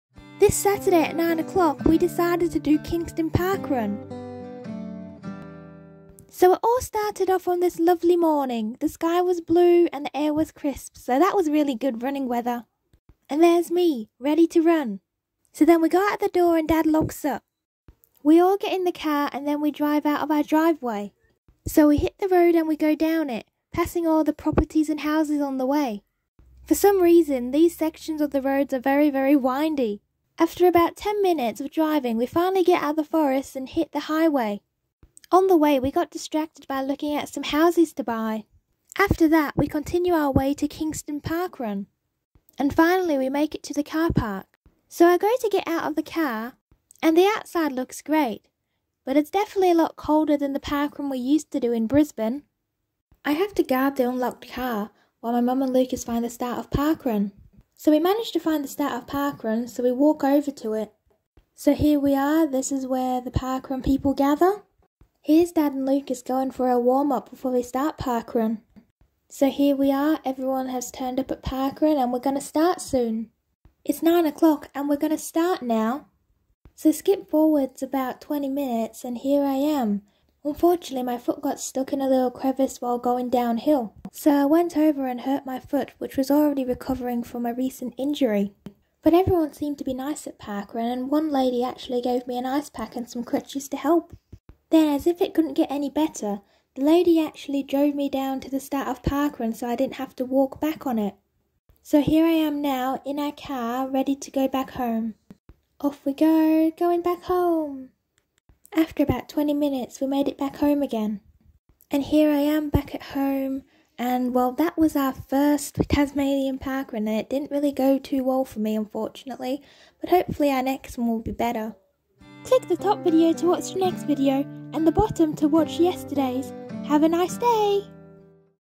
My Family And I Do Our First Kingston Parkrun 🏃‍♂️🏃‍♀️🏃
Hey guys, it's Saturday morning and this morning we're going to do Kingston Parkrun!
Mum, Dad, and Lucas did very well times! However, I managed to injure my foot by getting it stuck in a little crevice and falling down... But apart from that, everything was great!